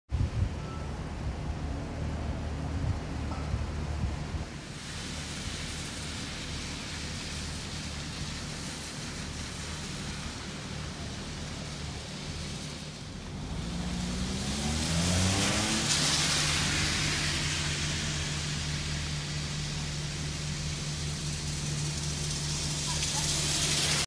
打ち水効果で消雪パイプを活用 新潟県三条市
昔ながらの涼を呼ぶ打ち水効果で熱中症対策にもなればと、新潟県三条市は8月2日、昨夏に続いて暑さ指数（WBGT）が31度以上となる日に消雪パイプを活用した打ち水を開始した。